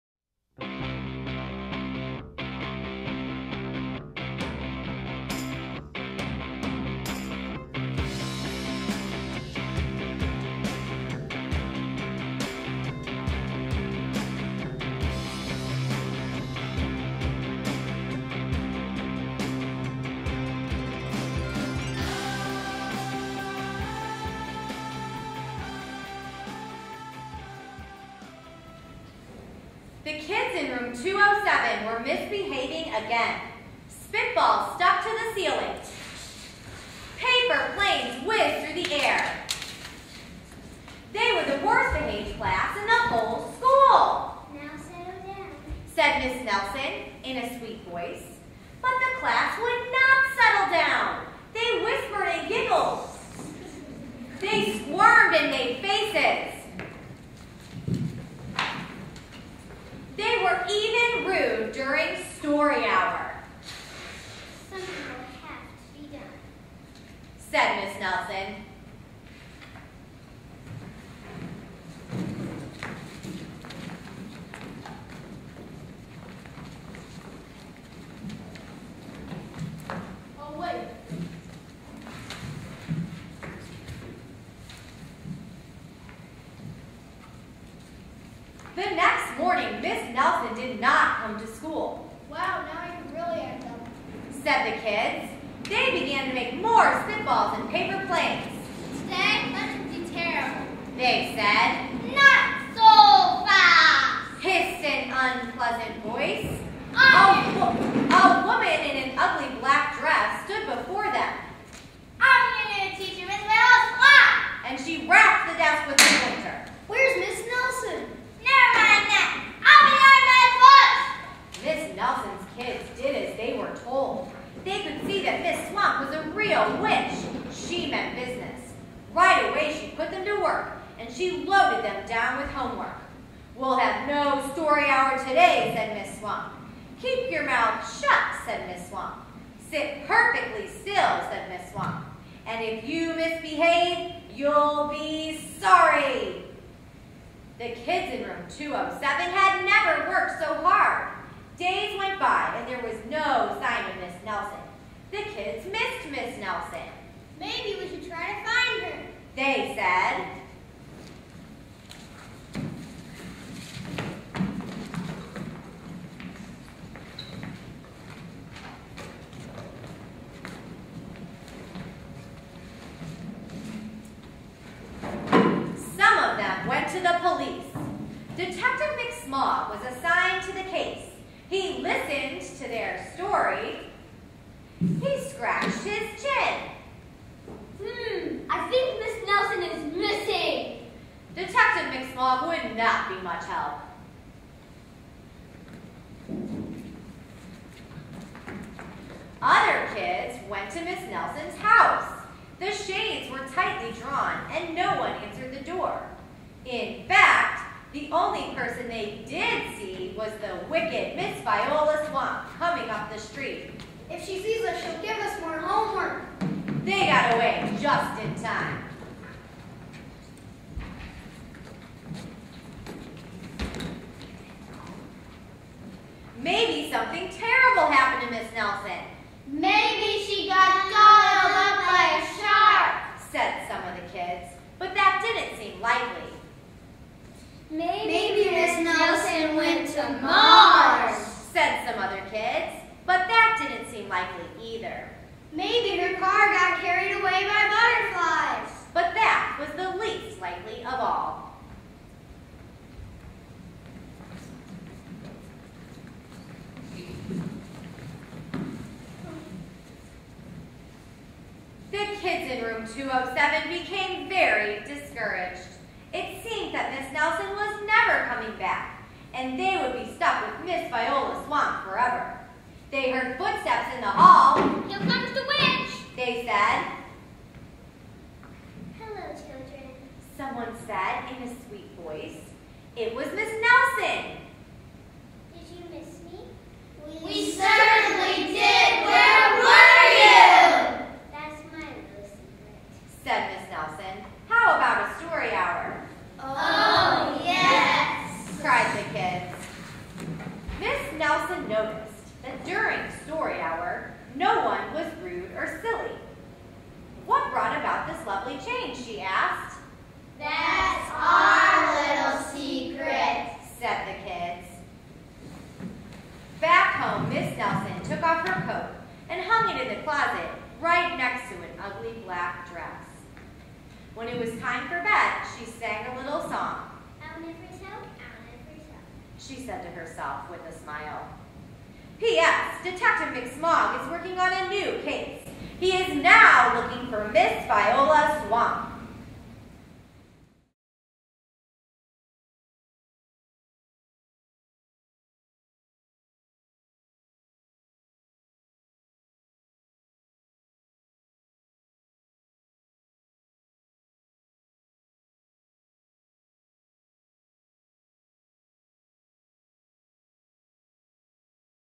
Ms. Nelson is Missing by LS 3/4
See Churchill's LS 3/4 acting out 'Ms. Nelson is Missing' as their teacher reads the book aloud.